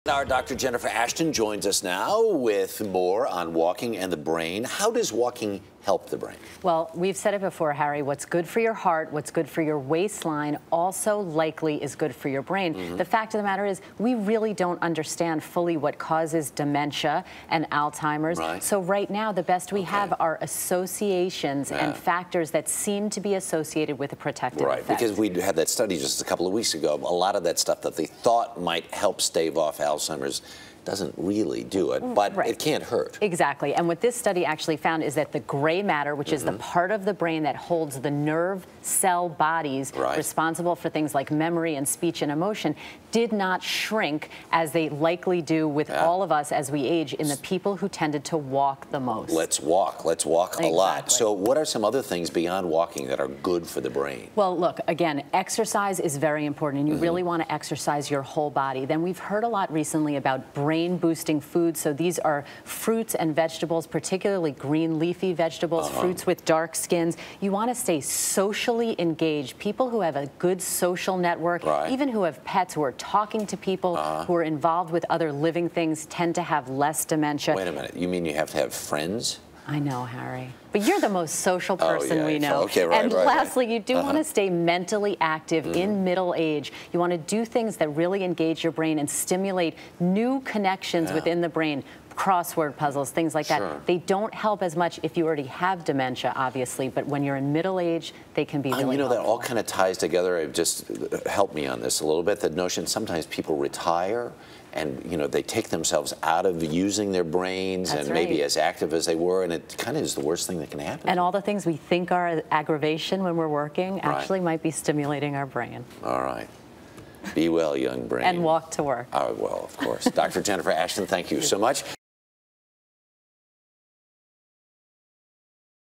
Walking for Your Brain
Harry Smith speaks with Dr. Jennifer Ashton about a study that says an active lifestyle can keep the brain healthy longer.